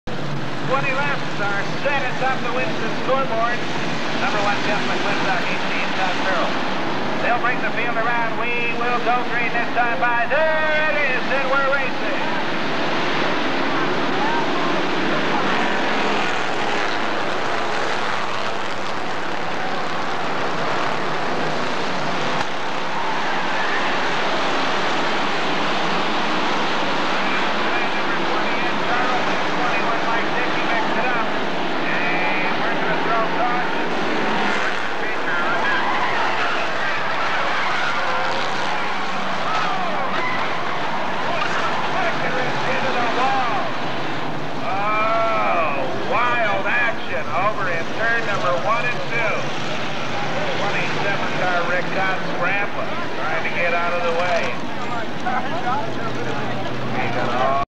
Elko Speedway 1994
Feature start for the Thundercar division.  Keep your eye on #21 Mike Dickey.  He got a good start, maybe too good(?), but things went down hill rapidly.